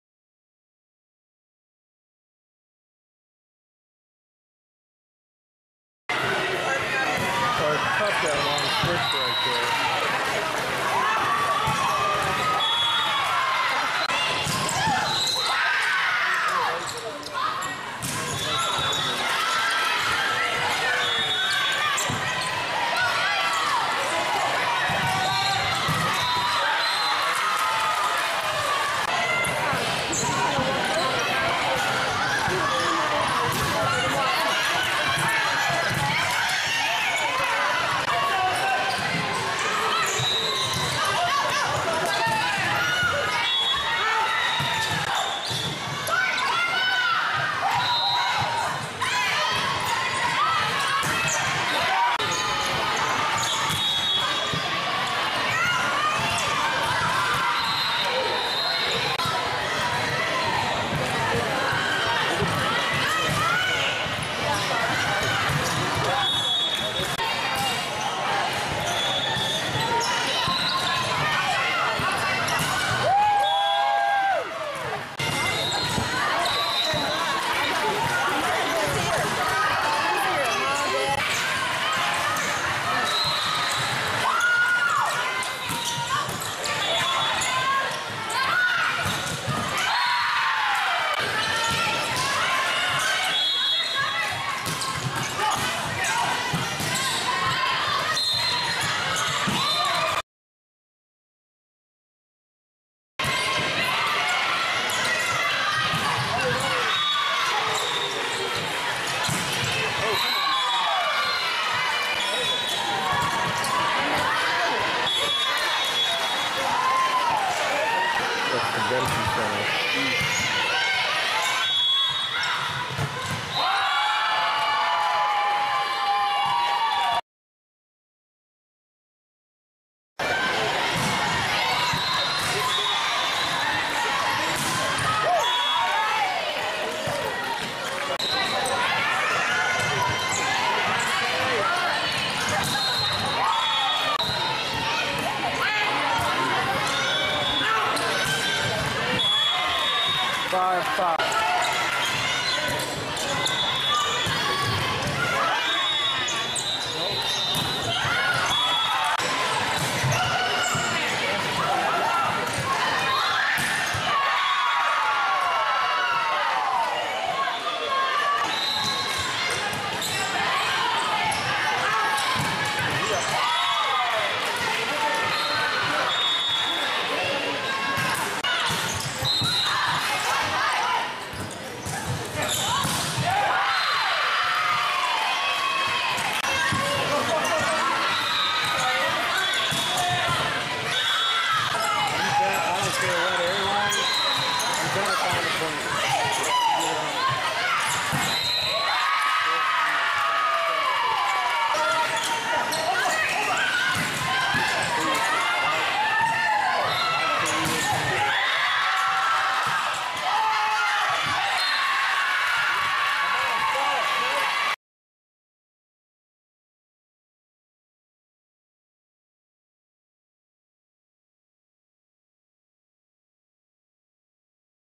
TOT Houston Finals Aniah Philo #8 TAV 17 Black Clip
Tour of Texas Finals Houston; TAV vs Mad Frog and Skyline Video Clips; Aniah Philo-Hatfield #8 OH-6 Rotational TAV 17 Black
H: 5'10"  VA:10'